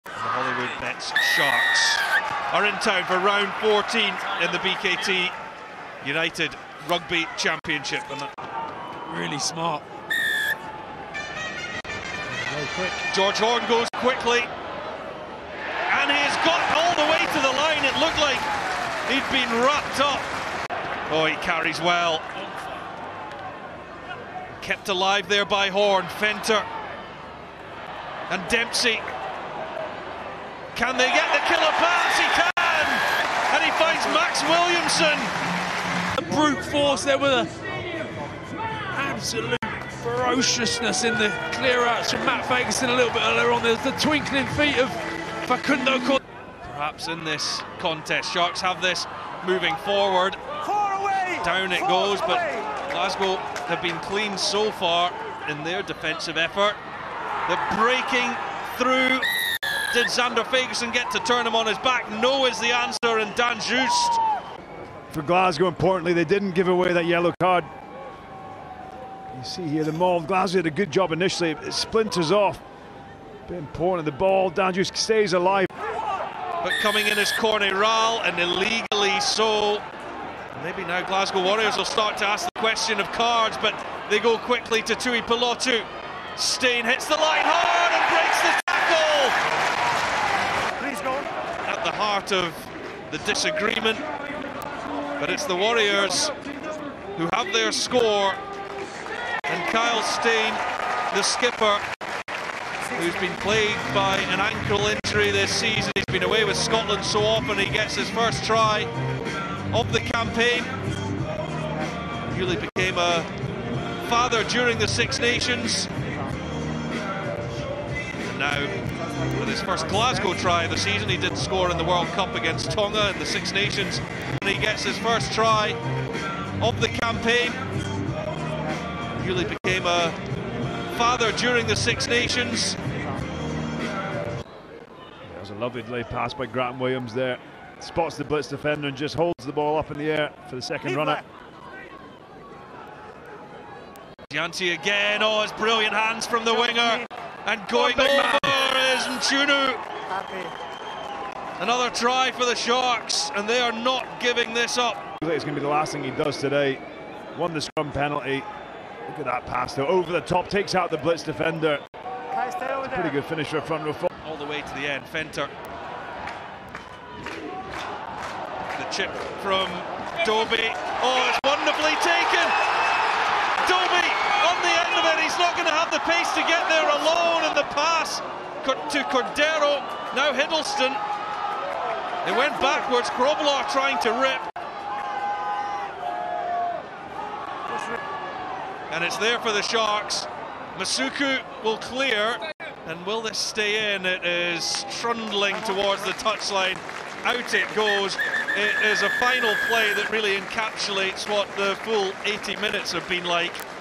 Glasgow Warriors v Hollywoodbets Sharks | Instant Highlights | Round 14 | URC 2023/24
Check out the highlights of the action as Glasgow Warriors took on Hollywoodbets Sharks in Round 14 of the BKT United Rugby Championship.

Head to URC TV to watch extended highlights of each game